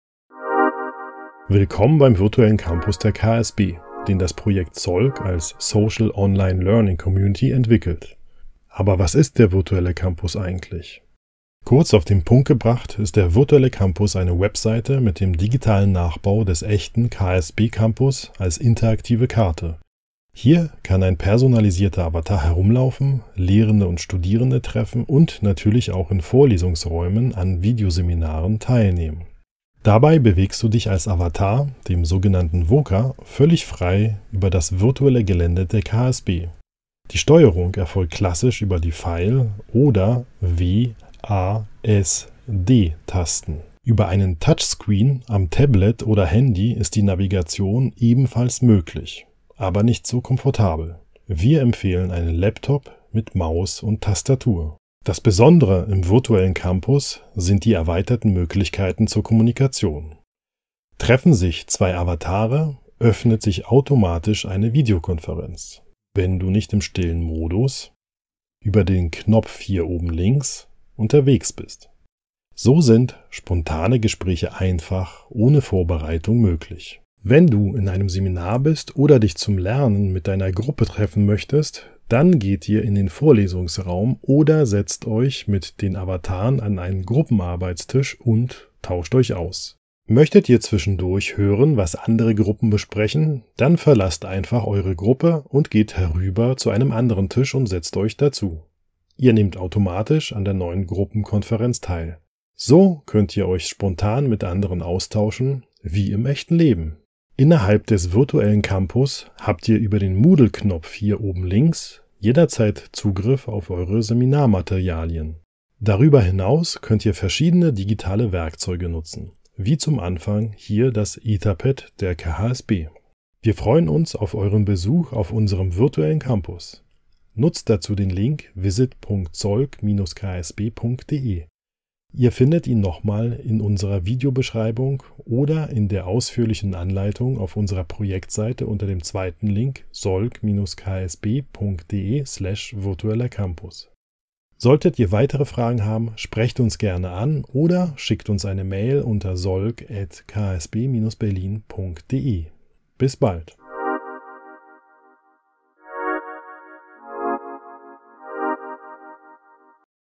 Virtueller KHSB Campus
Der Virtuelle Campus (VC) wird vom Projekt SOLC (Social Online Learning Community) an der KHSB (Katholische Hochschule für Sozialwesen Berlin) als interaktives Videokonferenzprogramm für Studierende getestet.

Er soll eine Alternative zu üblichen Videokonferenzprogramm anbieten, bei dem die Open Source Plattform WorkAdventure spontane Treffen sowohl für Studierende als auch für Lehrende ermöglicht und so vor allem für Gruppenarbeiten eine Erleichterung darstellen. Zusätzlich dient der VC auch als Schnittstelle zu Moodle und anderen an der KHSB eingesetzten digitalen Werkzeugen.